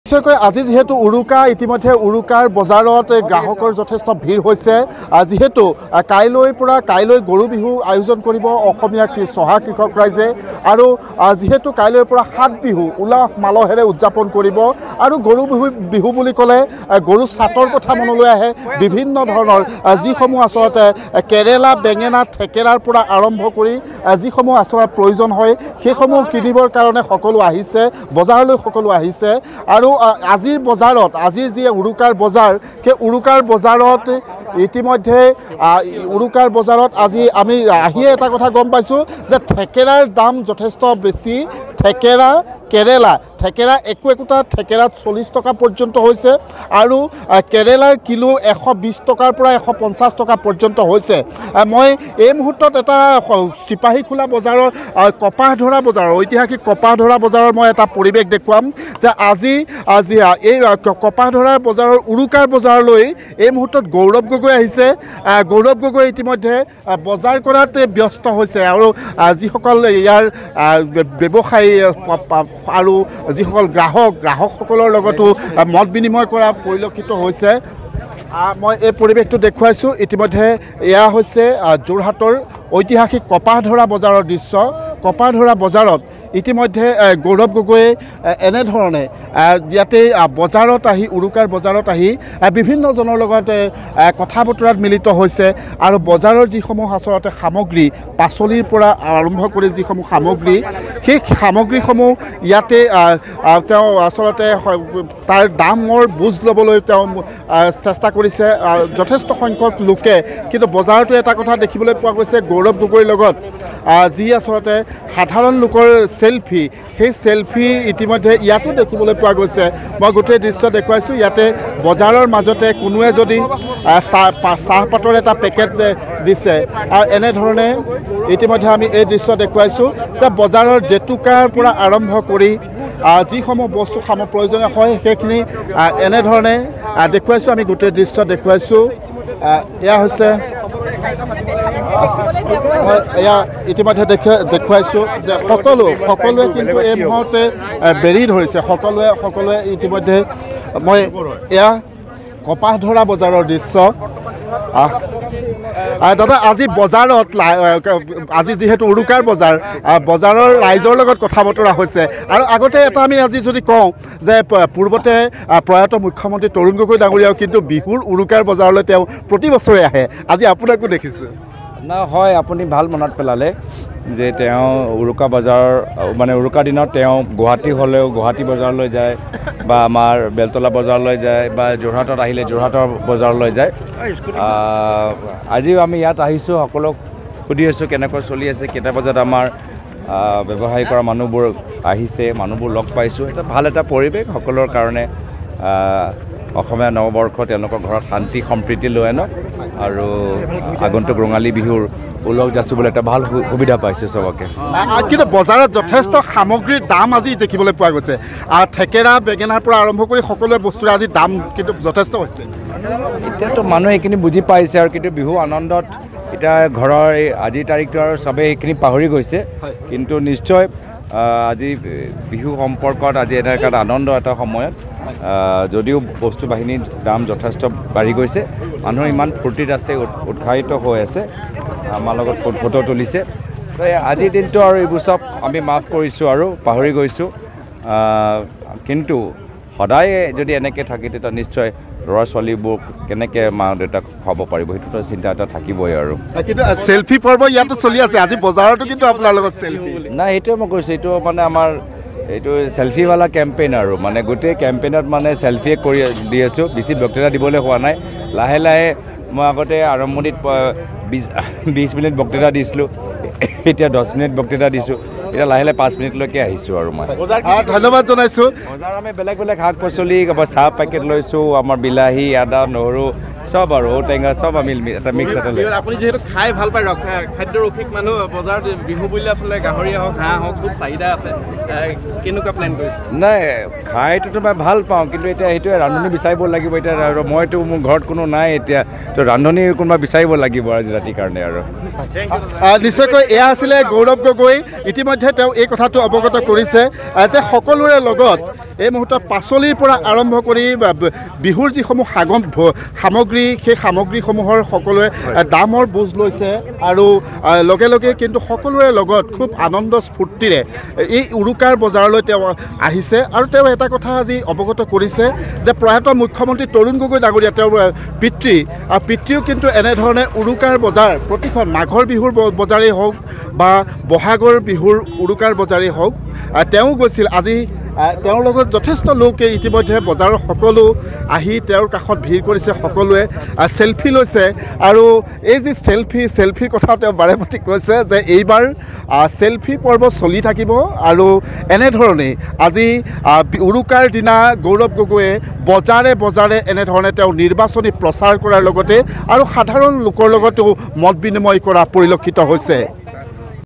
Gaurav Gogoi at Bihu Market : গৌৰৱ গগৈয়ে কিনিলে ঠেকেৰা, বেঙেনা, ঔটেঙা | N18V
Gaurav Gogoi at Bihu Market | আজি উৰুকা
# উৰুকাৰ বজাৰত গ্ৰাহকৰ ভিৰ
# যোৰহাটৰ কপাহধৰা বজাৰত গৌৰৱ গগৈয়ে কিনিলে ঠেকেৰা, বেঙেনা, ঔটেঙা
# গৌৰৱ গগৈৰ লগত বজাৰতো দেখিবলৈ পোৱা গল চেলফি পৰ্ব
#  কপাহধৰা বজাৰৰ পৰাই সকলোকে জনালে বিহুৰ শুভেচ্ছা